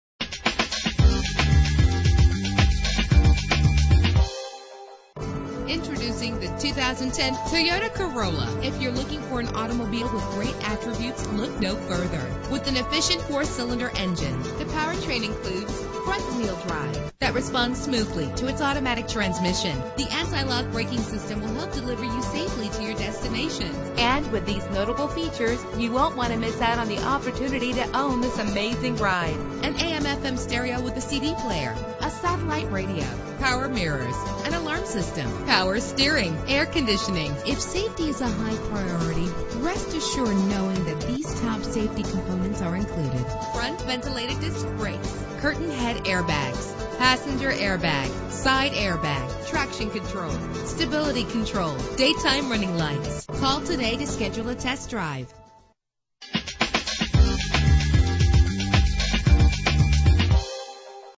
2010 Toyota Corolla - Raleigh NC
Year: 2010
Make: Toyota
Model: Corolla
Trim: S
Engine: 1.8 liter inline 4 cylinder 16 valve
Transmission: 4-Speed Automatic
Color: Blue
Mileage: 43556
Address:
9101 Glenwood Avenue